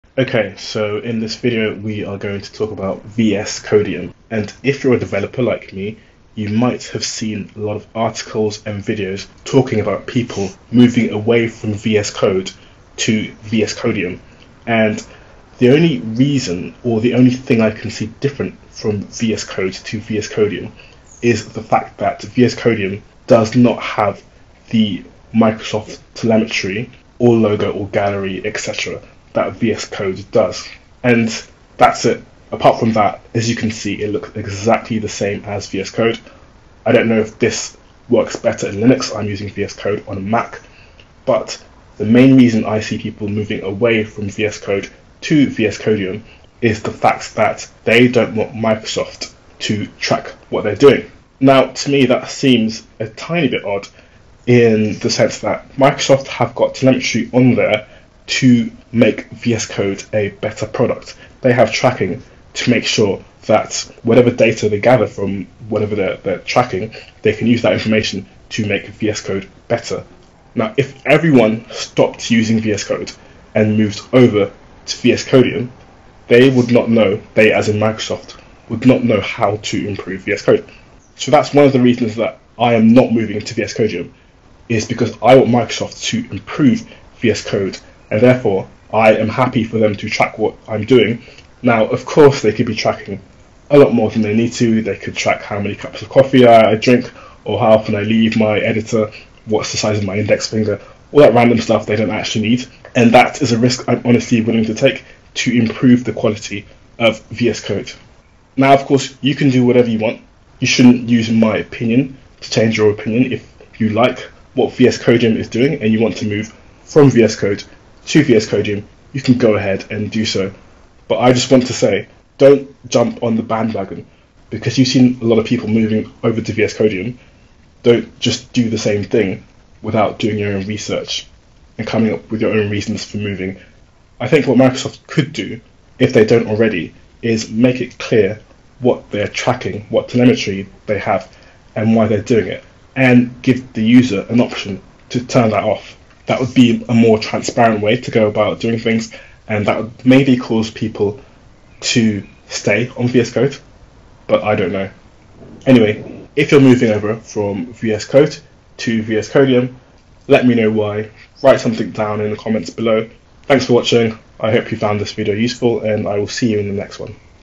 Why I’m not moving to from VSCode to VSCodium
VSCodium seems to be the hot new IDE right now but why? It's not really different from VSCode and I personally don't see the need to switch. I go through why in this video.